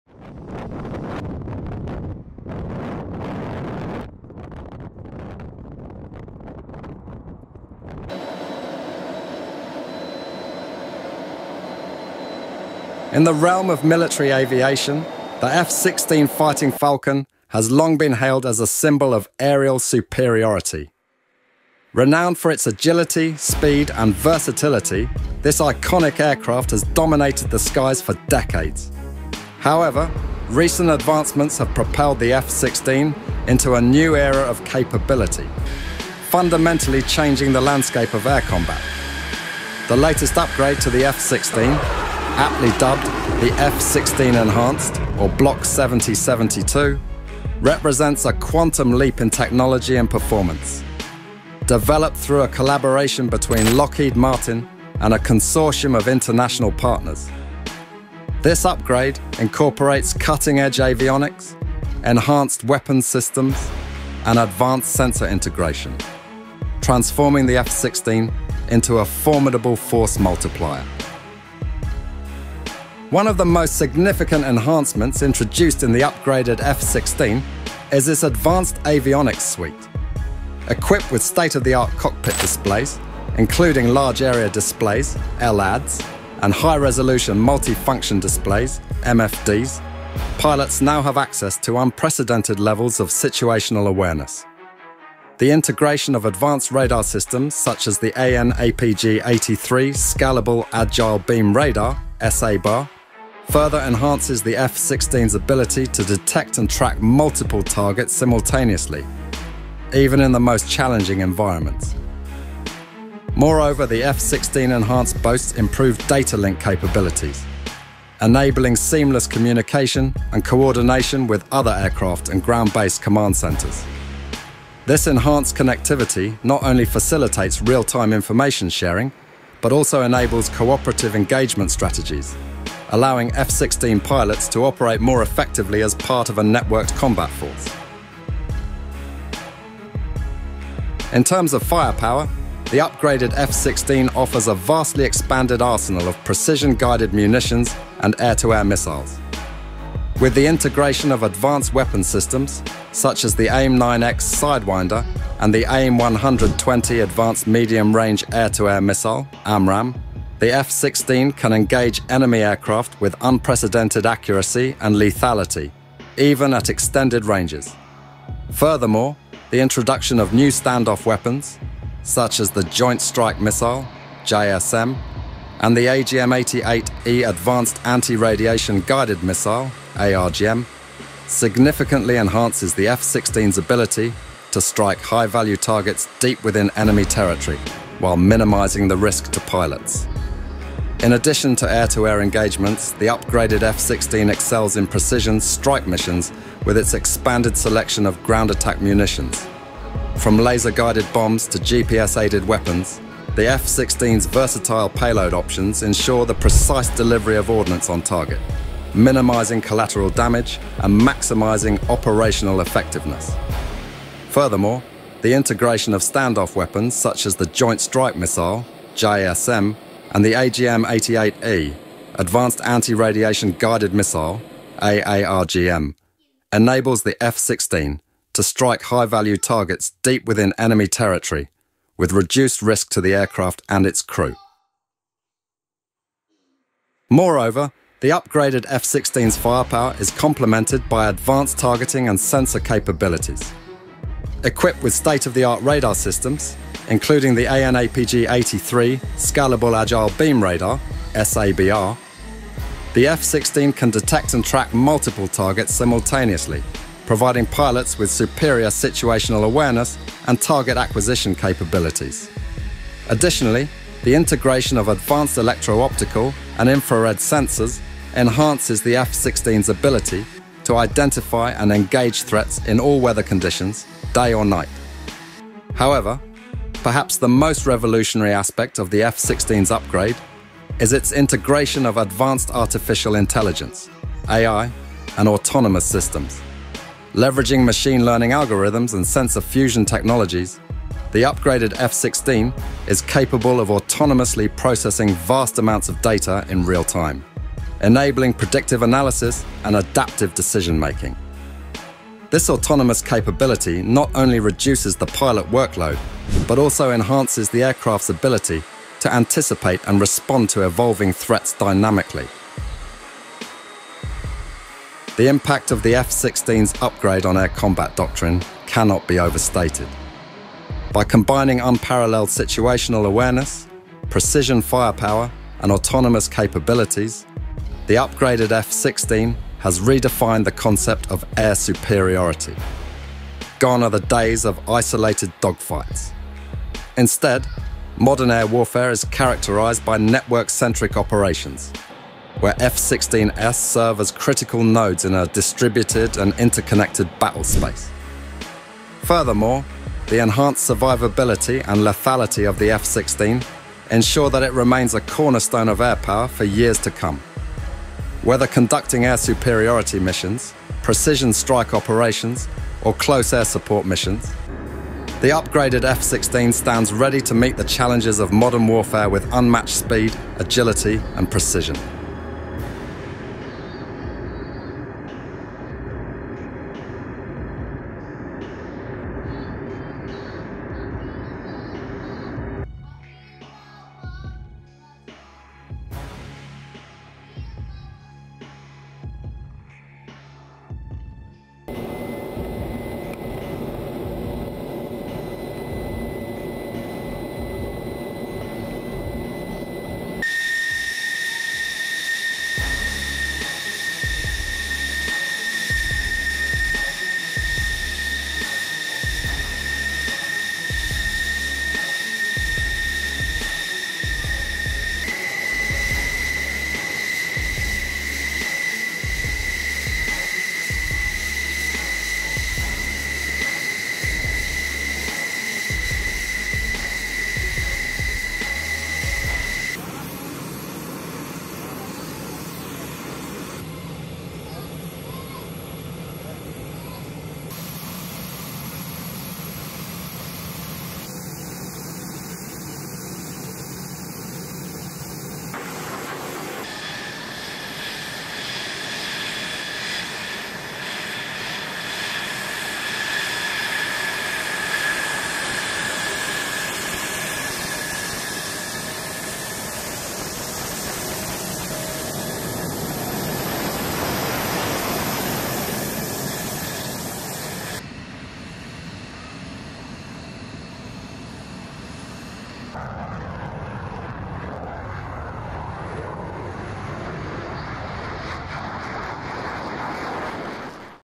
Unveiling the F-16's Revolutionary Upgrade: Changing Air Combat Forever
In the realm of military aviation, the F-16 Fighting Falcon has long been hailed as a symbol of aerial superiority. Renowned for its agility, speed, and versatility, this iconic aircraft has dominated the skies for decades. However, recent advancements have propelled the F-16 into a new era of capability, fundamentally changing the landscape of air combat.